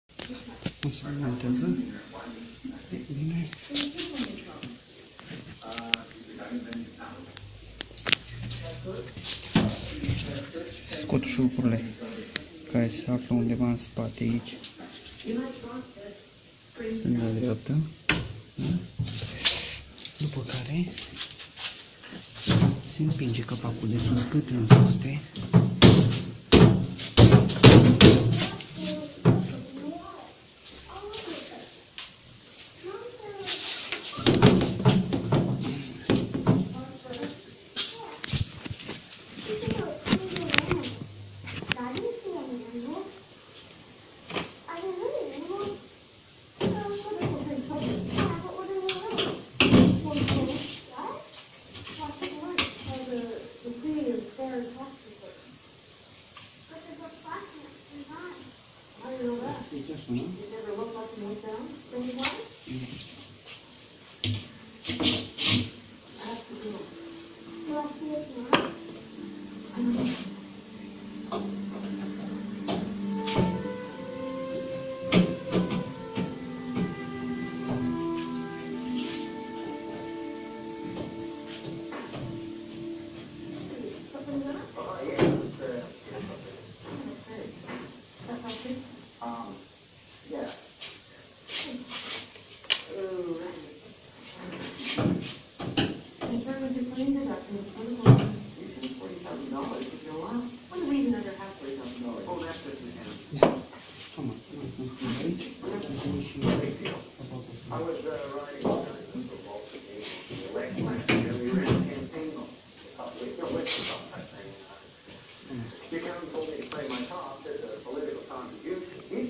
Demontare capac masina de spalat Indesit WIE87
Daca va avea careva nevoie sa desfaca capacul de la masina de spalat rufe Indesit WIE87, am pus o inregistrare aici, cu speranta ca va  va fi mai usor sa o faceti. Desfaceti suruburile din spate, apoi impingeti capacul sa culiseze putin, dupa care se ridica.